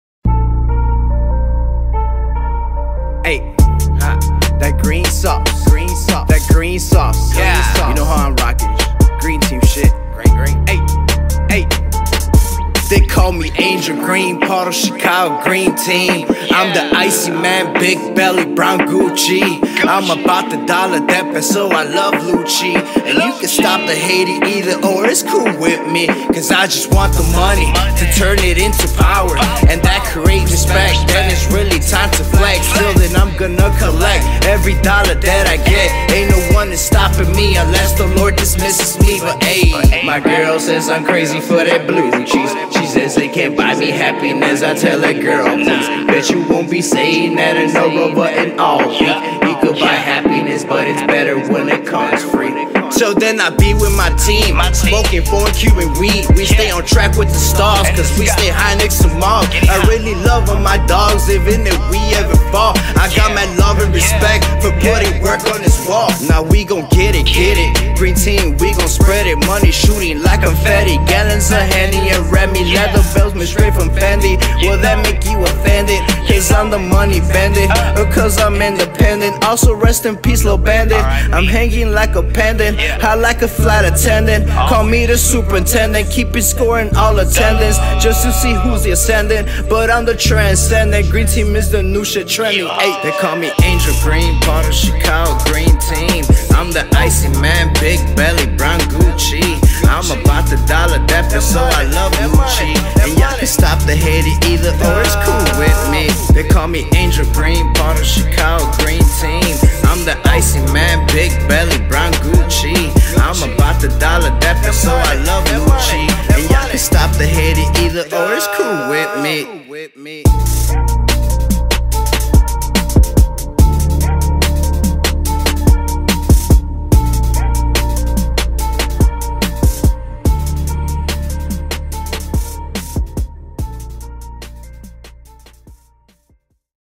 Angel Green x Green Doze
Angel Green from Chicago has launched a new hit. Follow the trend. Listen to good music with good vibes.